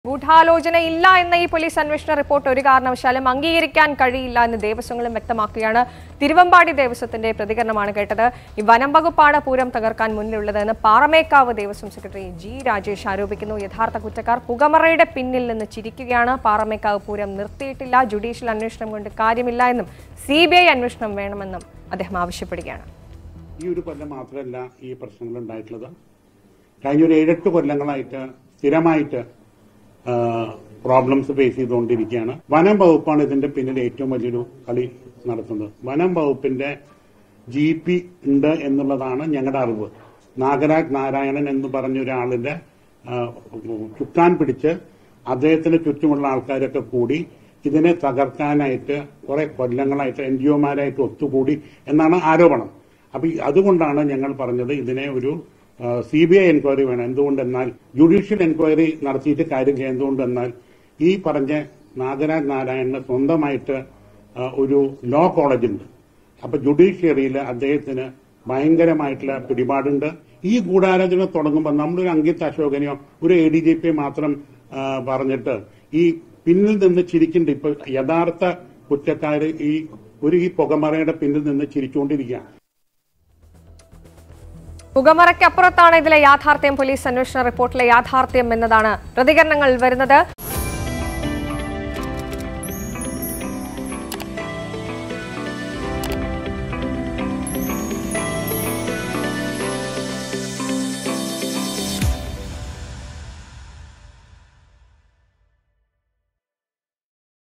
'പൂരം തകർക്കാൻ മുന്നിലുള്ളത് വനംവകുപ്പ്' ; പാറമേക്കാവ് ദേവസ്വം
'പൂരം തകർക്കാൻ മുന്നിലുള്ളത് വനംവകുപ്പ്,സിബിഐ അന്വേഷണം വേണം  ' ; പാറമേക്കാവ് ദേവസ്വം